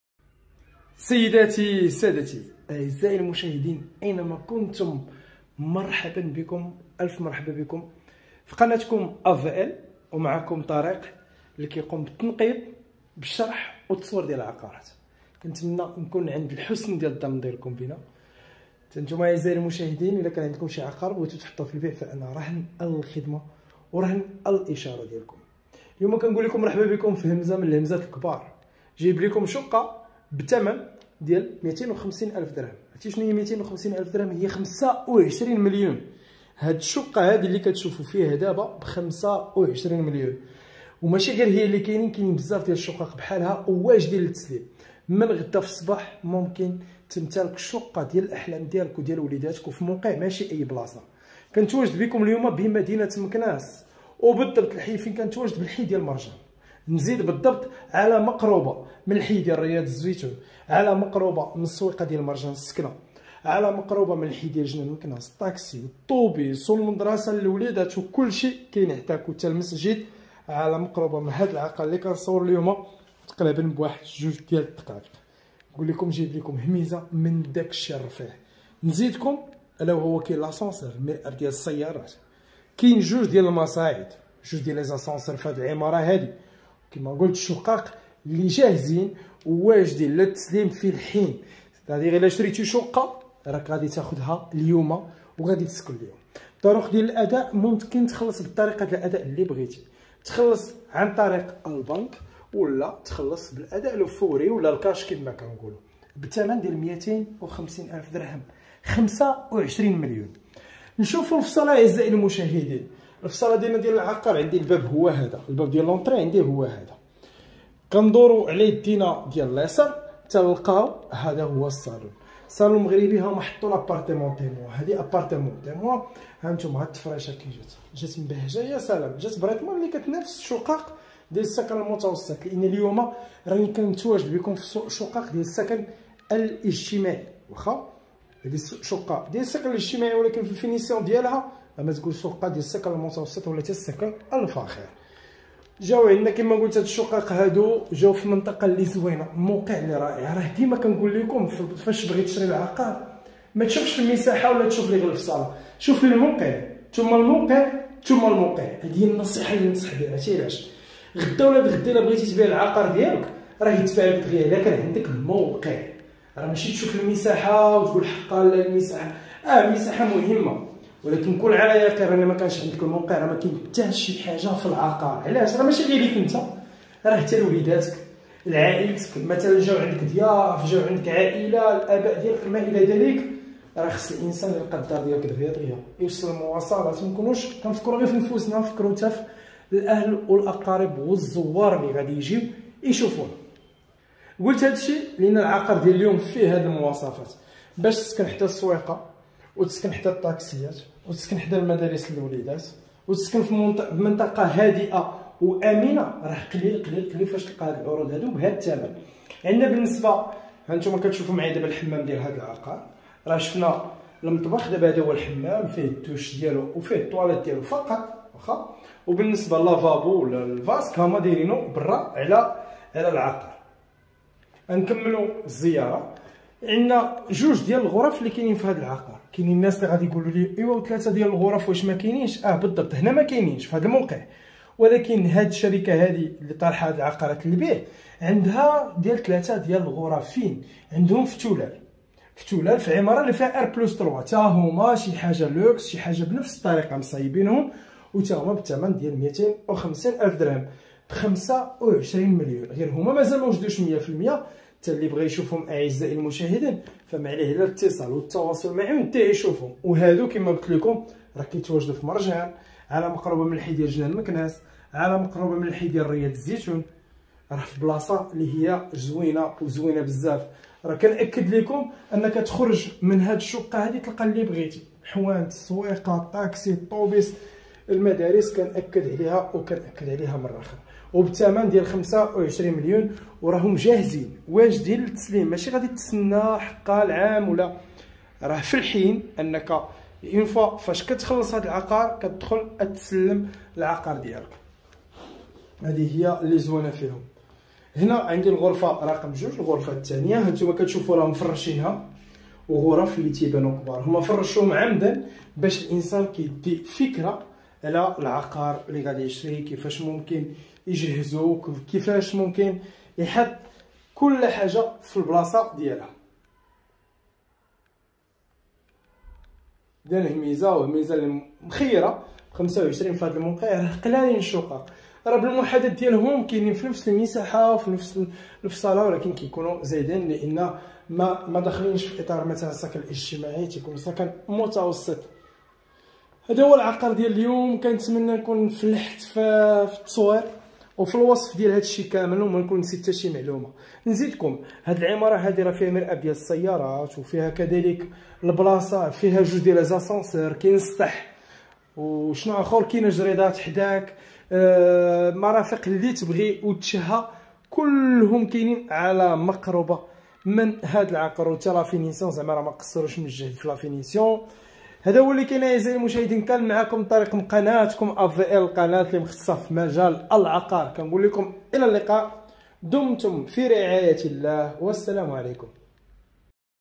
مزال التخفيض كاين🥸شقق دبا ب 25 مليون😭Appartement 250000dh meknes
هادي همزة ماتلقاوش بحالها 💛شقة سكن إجتماعي 💛اوا استغل الفرصةوتمتع بهاد العقار لي قليل فالسوق💛